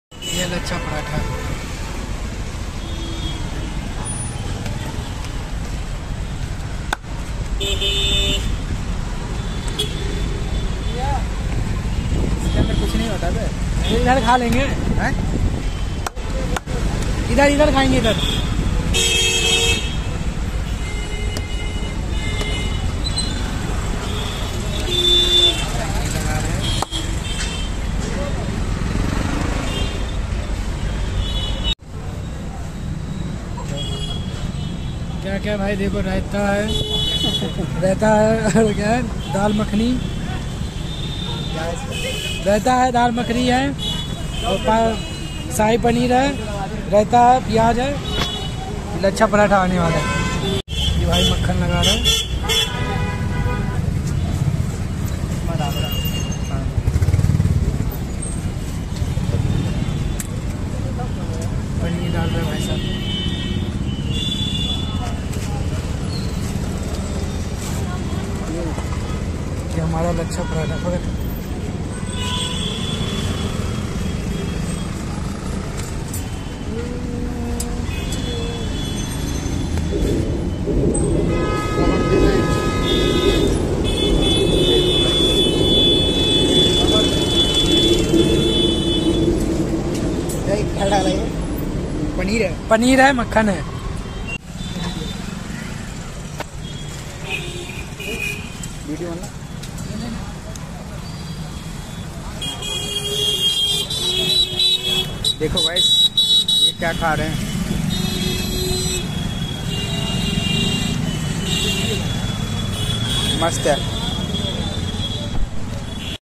😋achha paratha. dal makhani. shahi paneer.. raita.. 🍱.. thali ₹60.. . Sikandarpur ghosi.. 📍.. 😍😲
😋achha paratha. dal makhani. shahi paneer.. raita.. 🍱.. thali ₹60.. . Sikandarpur ghosi.. 📍.. 😍😲.. .. what tasty lachha paratha..